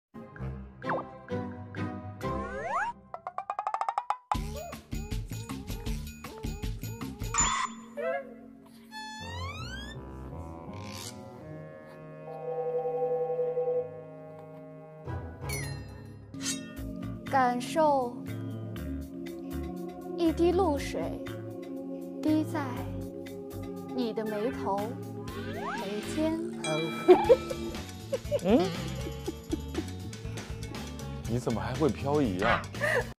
Shen Yue sneaked in to complete the task, but was caught by DiGe.
เสิ่นเยว่ แอบทำภารกิจให้เสร็จ แต่ถูกหยางตี๋จับได้